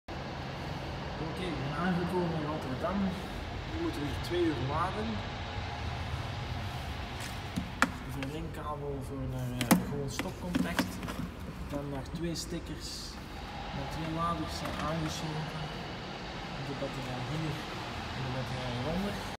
Ezra charging up the NIU N-Pro at Coolsingel Rotterdam
This video is part of the latest blog, written by Ezra van Manen translated by me. Read the whole story about the trip from Antwerp to Amsterdam